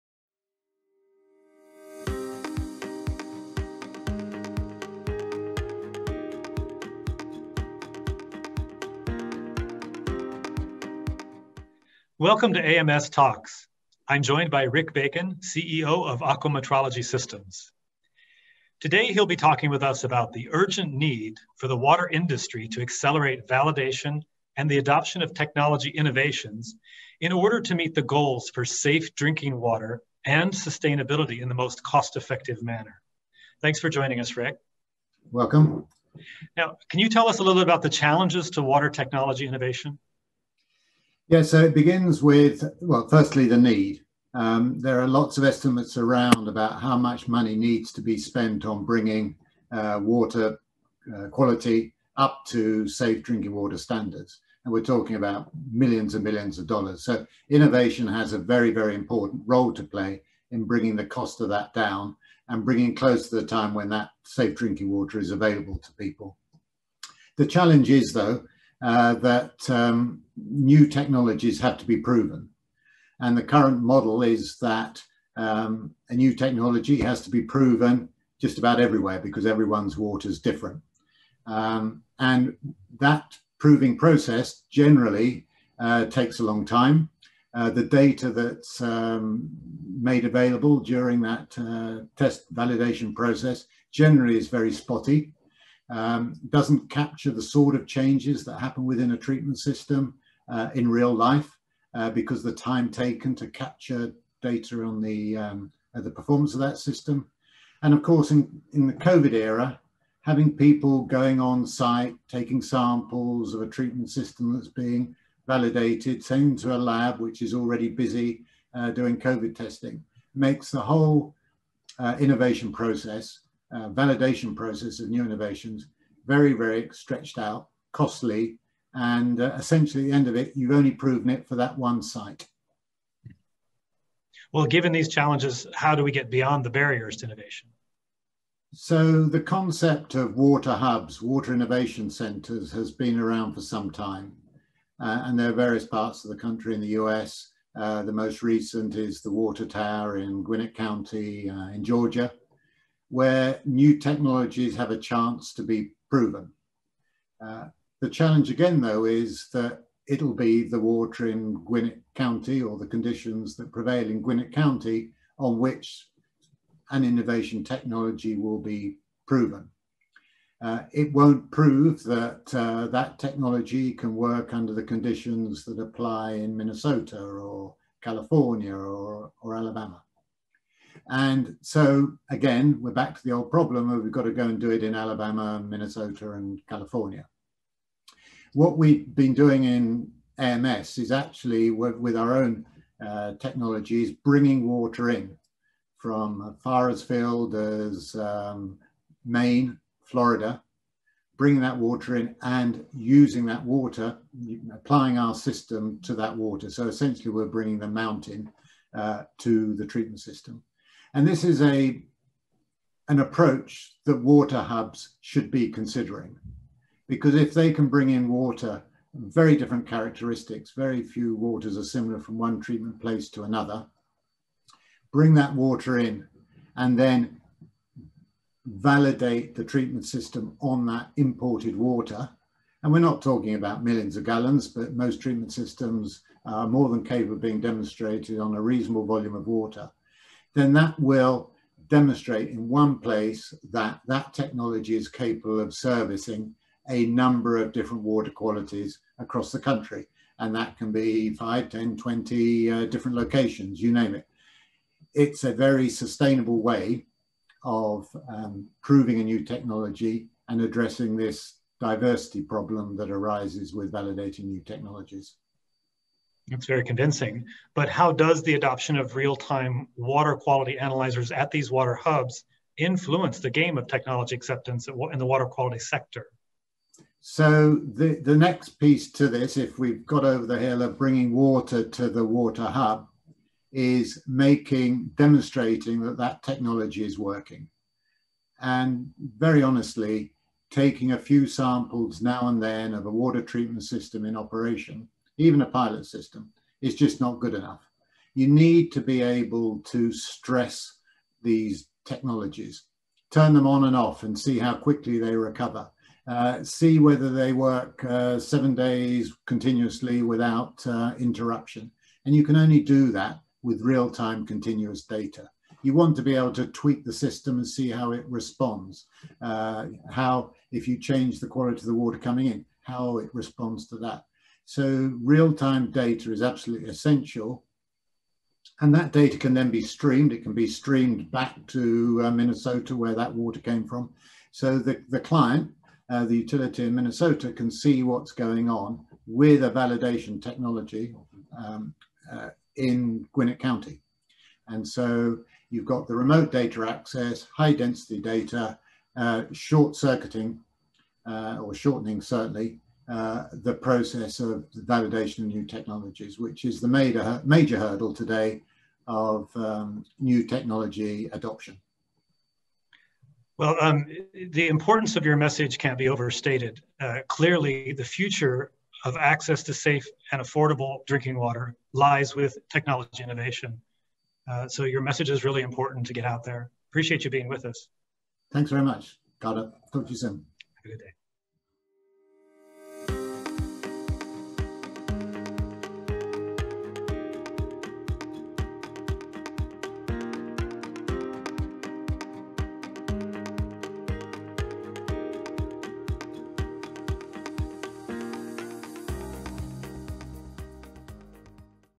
AMS Talks: Accelerating Validation and Adoption of Technology Innovations
In this episode of AMS Talks, Rick Bacon, CEO of AMS, discusses the urgent need for the water industry to accelerate validation and the adoption of technology innovations in the water industry in order to meet the goals for safe drinking water and sustainability in the most cost-effective manner.